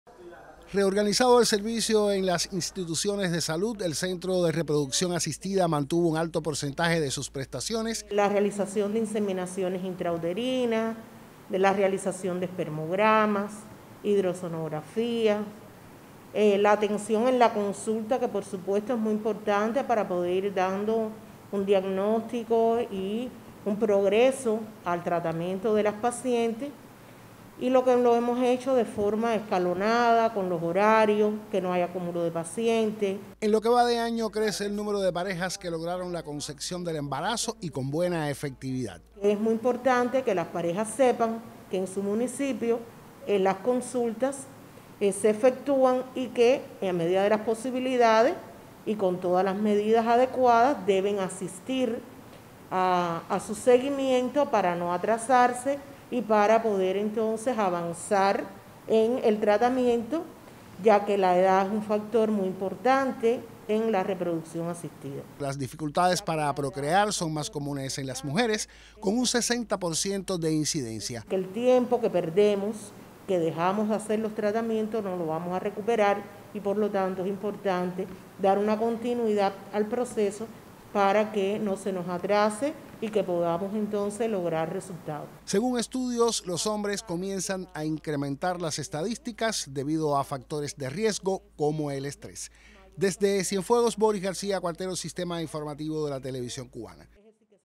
Mantiene servicios Centro de Reproducción Asistida de Cienfuegos
El Centro de Reproducción Asistida en Cienfuegos, ha mantenido los servicios que presta a parejas con dificultades para concebir embarazos. Solo restringió las consultas a pacientes de otras provincias para propiciar la restricción de movilidad impuesta en el enfrentamiento a la pandemia.